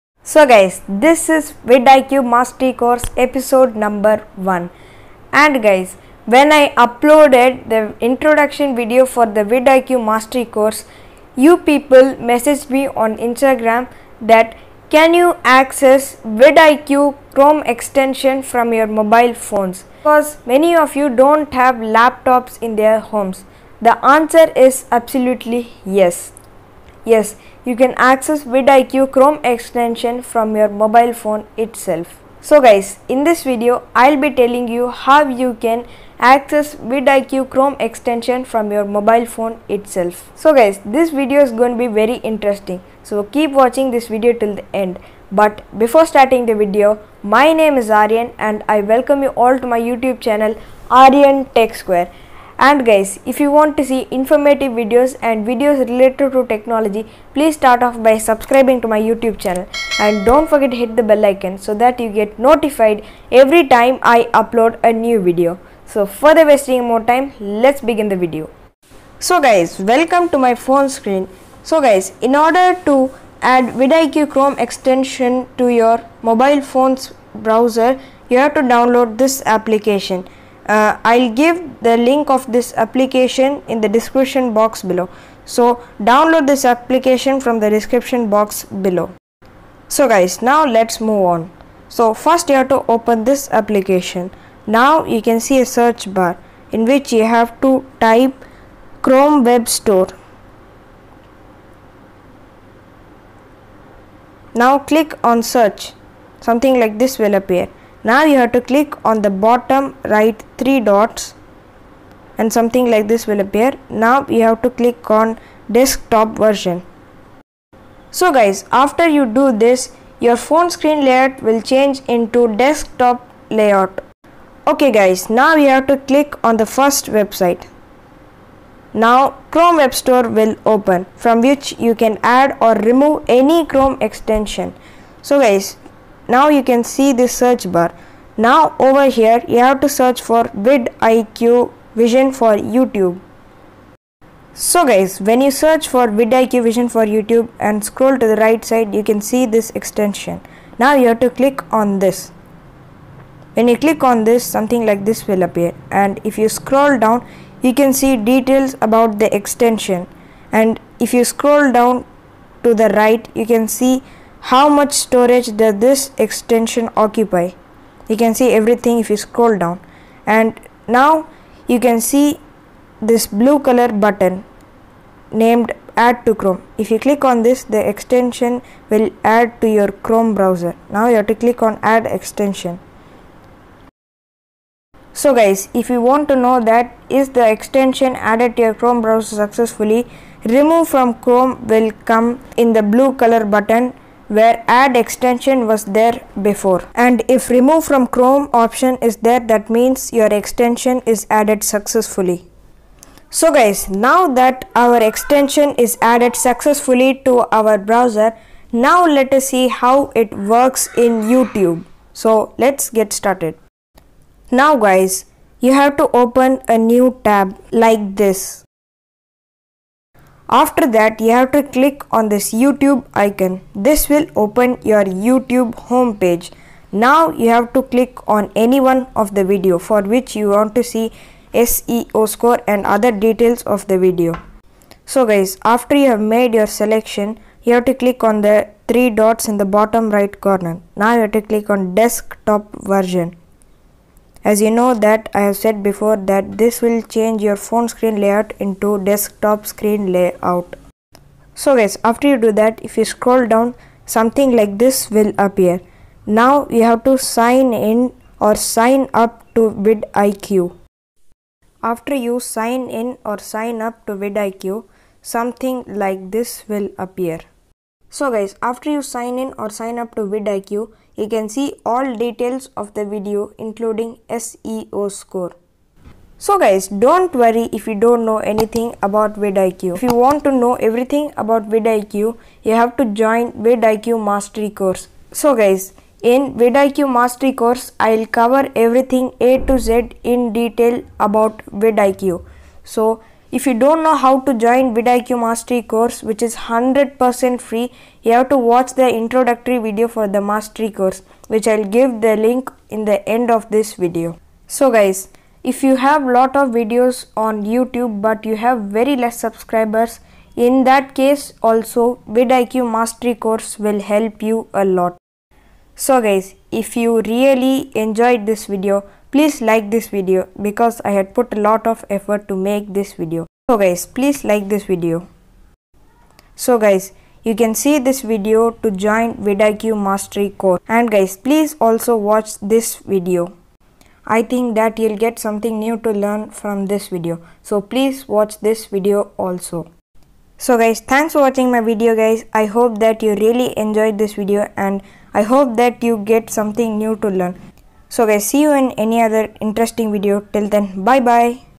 How To Install Vidiq Extension On Mobile | How To Use Vidiq | 2021
How To Install Vidiq Chrome Extension In Mobile | Vidiq Extension For Mobile | How To Use Vidiq |

so guys in this video I will show you guys how you can add vidiq chrome extension to your mobile phone browser. how you can use vidiq on mobile